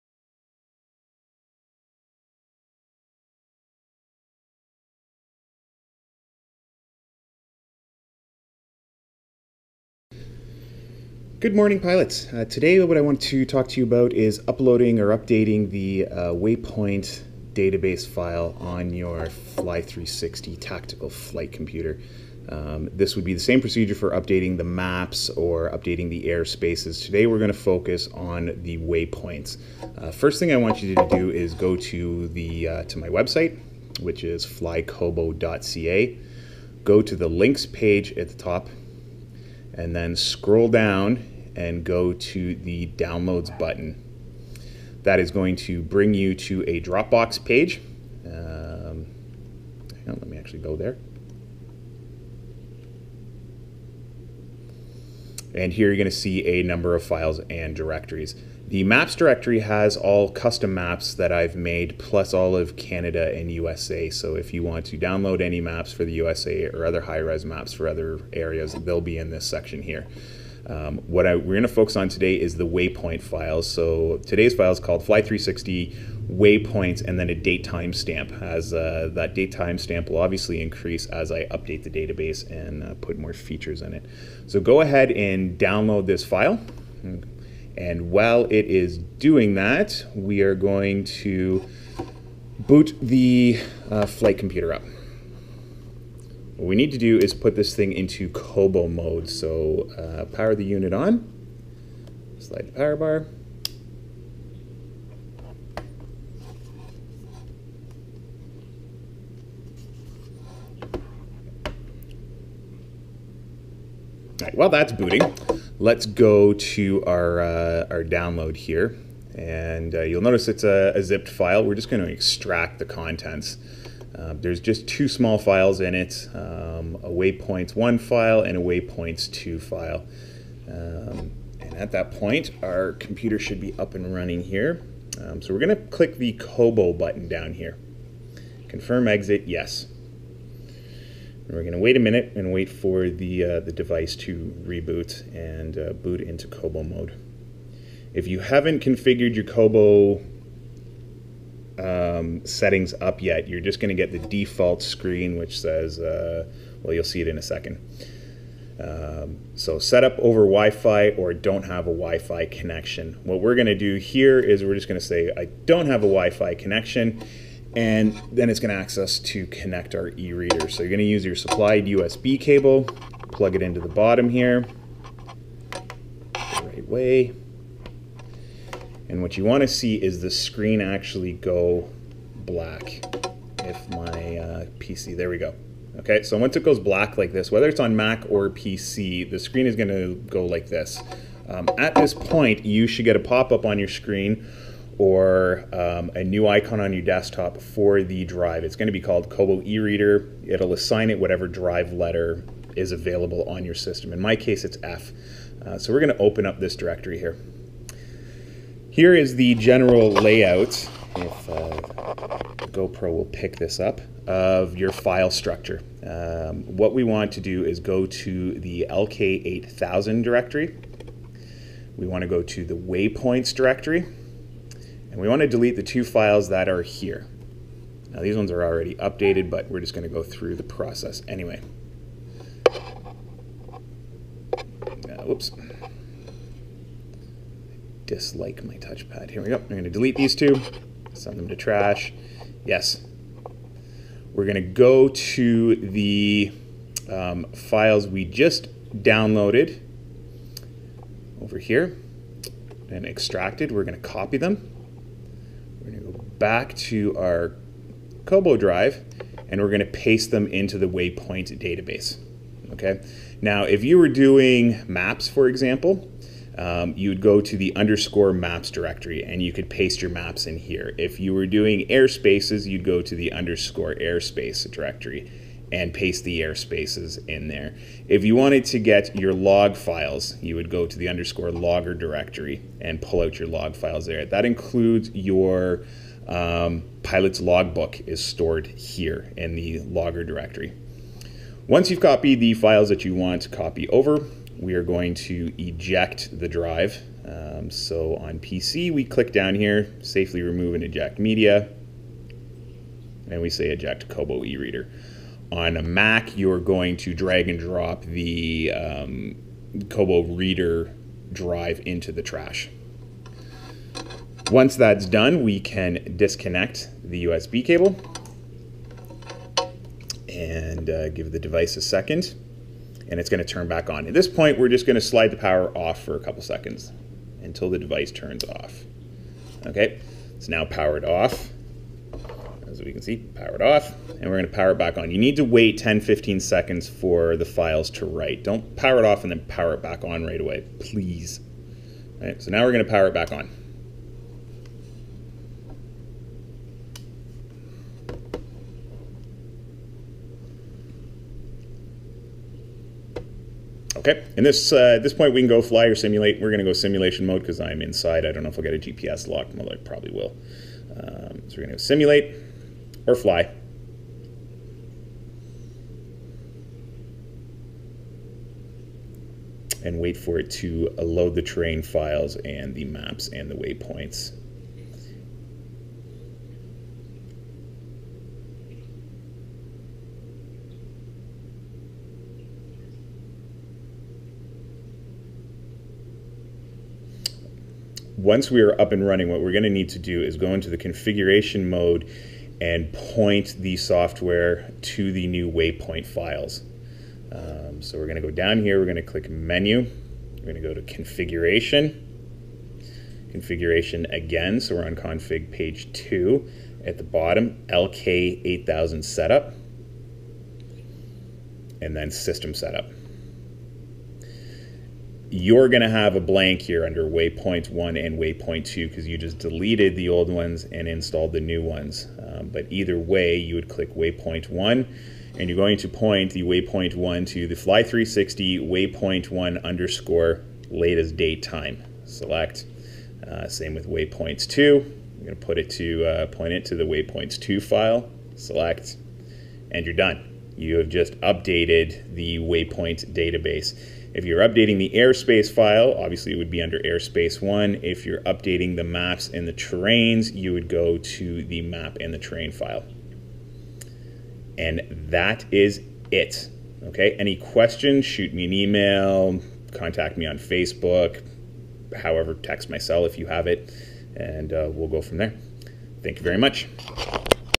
Fly360 LK8000 Updating Waypoints DO NOT SETUP OVER WIFI
How to update the Waypoints, Maps, Airspaces on your Fly360
Please Note DO NOT SETUP OVER WIFI.  Just Click No Wifi and Connect the USB Cable